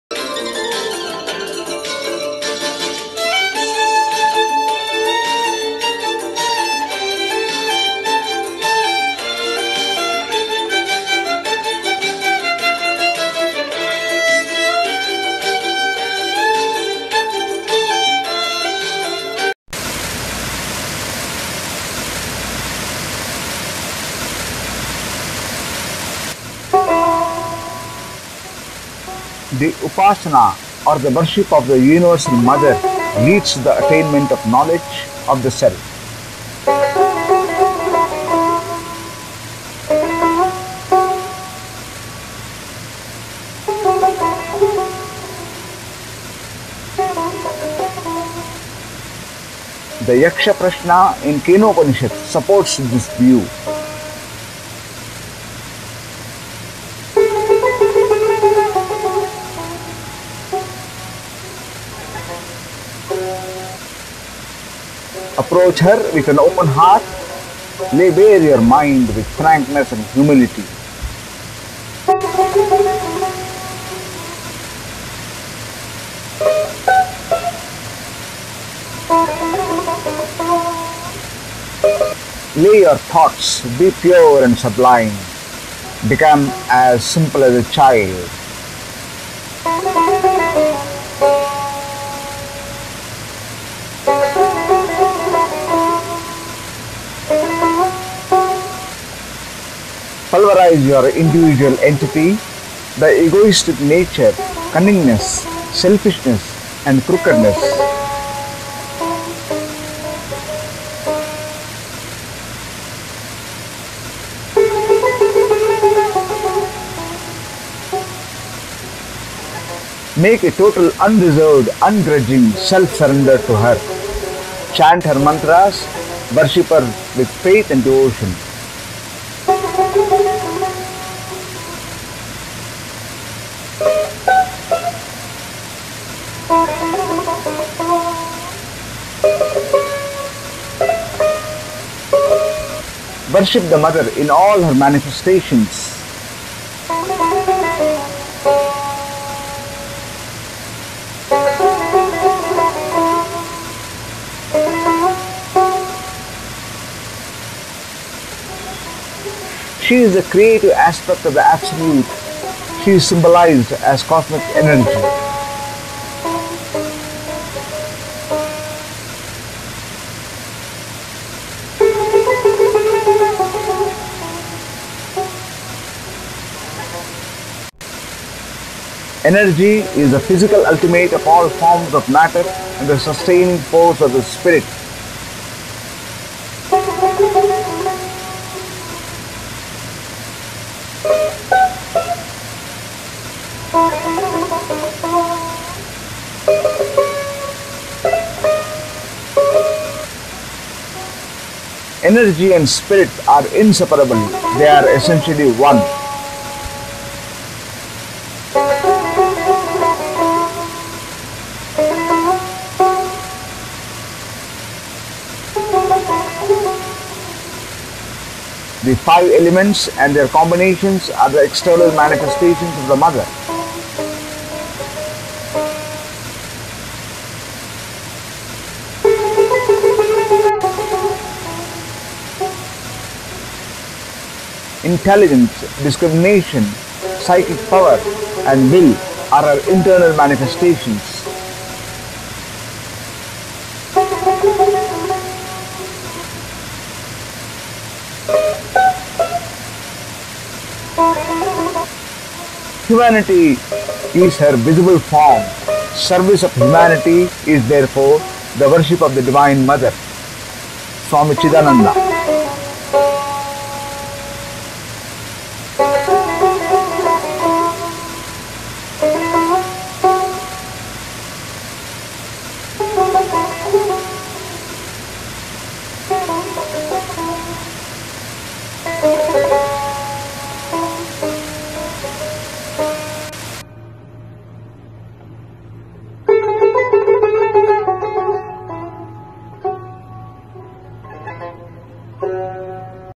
SWAMI CHIDANANDA ANSWERS - 5
The Upasana or the worship of the Universal Mother leads to the attainment of knowledge of the Self.

The Yaksha Prasna in Kenopanishad supports this view.

Approach Her with an open heart. Lay bare your mind with frankness and humility.

Lay your thoughts be pure and sublime. Become as simple as a child.

Pulverise your individual entity, the egoistic nature, cunningness, selfishness and crookedness.

 Make a total, unreserved, ungrudging self-surrender to Her. Chant Her Mantras. Worship Her with faith and devotion.
Worship the Mother in all Her manifestations.

She is the creative aspect of the Absolute. She is symbolised as Cosmic Energy.

Energy is the physical ultimate of all forms of matter and the sustaining force of the Spirit.

 Energy and Spirit are inseparable. They are essentially one.

The five elements and their combinations are the external manifestations of the Mother.

Intelligence, discrimination, psychic power and will are Her internal manifestations.

Humanity is Her visible form. Service of humanity is, therefore, the worship of the Divine Mother.

Swami Chidananda.

Source credit:
by Swami CHIDANANDA